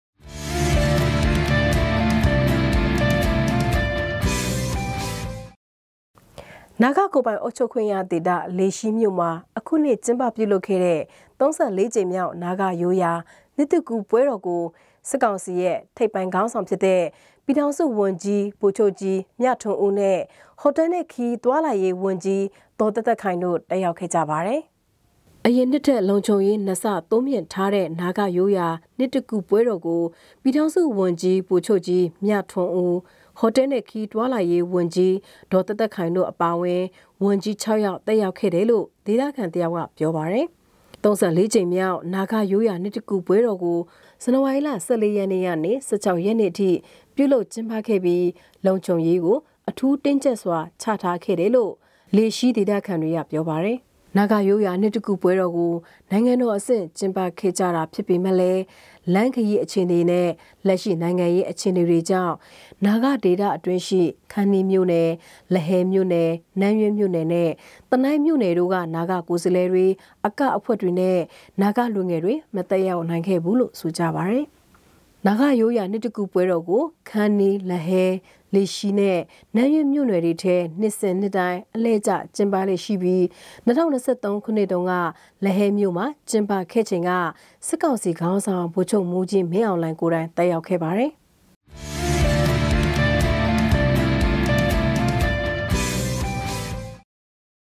လုံခြုံရေးတင်းကြပ်ထားသည့် နာဂနှစ်သစ်ကူးပွဲတော်ကို ဗိုလ်ချုပ်ကြီးမြထွန်းဦးနှင့် ဒေါ်သက်သက်ခိုင်တက်
နာဂကိုယ်ပိုင်အုပ်ချုပ်ခွင့်ရဒေသ လေရှီးမြို့မှာ အခုနှစ်ကျင်းပပြုလုပ်ခဲ့တဲ့ ၃၄ ကြိမ်မြောက် နာဂရိုးရာနှစ်သစ်ကူးပွဲတော်ကို စစ်ကောင်စီရဲ့ ထိပ်ပိုင်းခေါင်းဆောင်ဖြစ်တဲ့ ပြည်ထောင်စုဝန်ကြီး ဗိုလ်ချုပ်ကြီး မြထွန်းဦးနဲ့ ဟိုတယ်နဲ့ခရီးသွားလာရေးဝန်ကြီး ဒေါ်သက်သက်ခိုင်တို့ တက်ရောက်ခဲ့ကြပါတယ်။